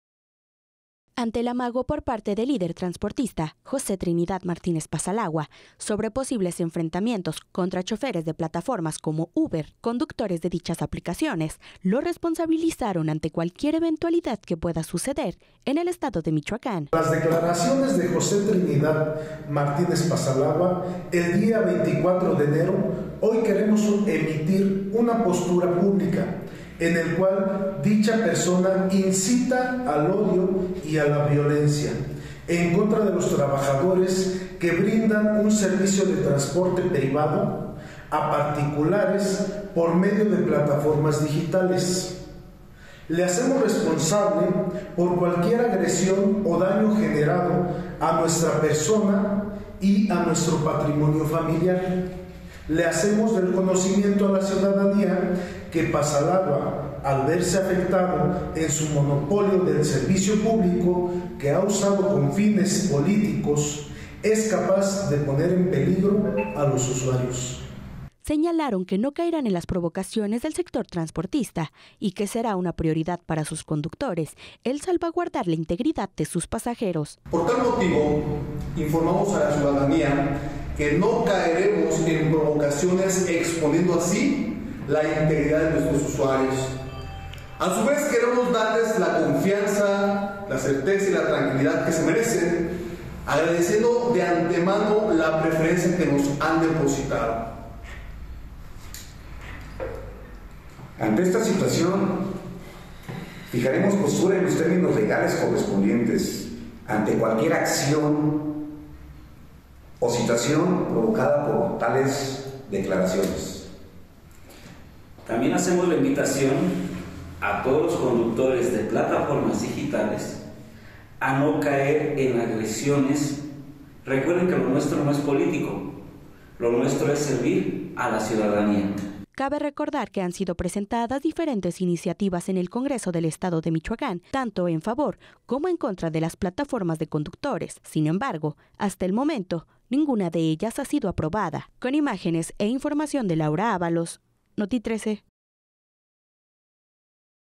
Conductores de plataformas digitales responsabilizan a José Trinidad Martínez por cualquier daño
Estatal 🔴 Conductores de plataformas digitales responsabilizan al líder transportista, José Trinidad Martínez Pasalagua, de cualquier daño a su integridad, piden no se difundan discursos de odio.

Laura Avalos | Notitrece